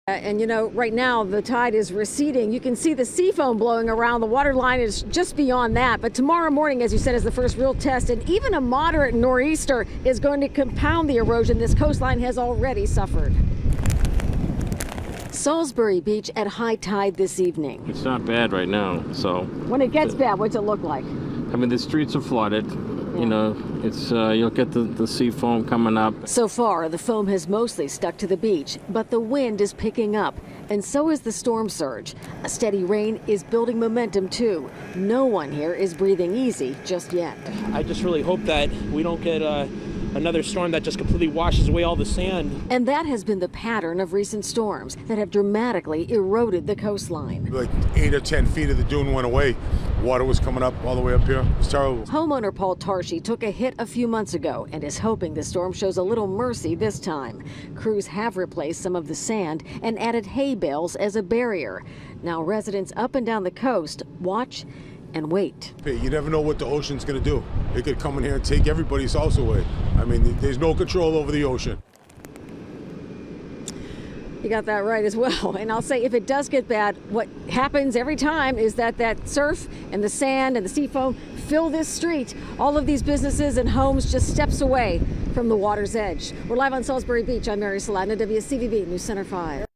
Nor'easter creates flooding concerns for those who live along Mass. coast
The wind is picking up along with the storm surge, and that along with steady rainfall is making people who have waterfront homes uneasy.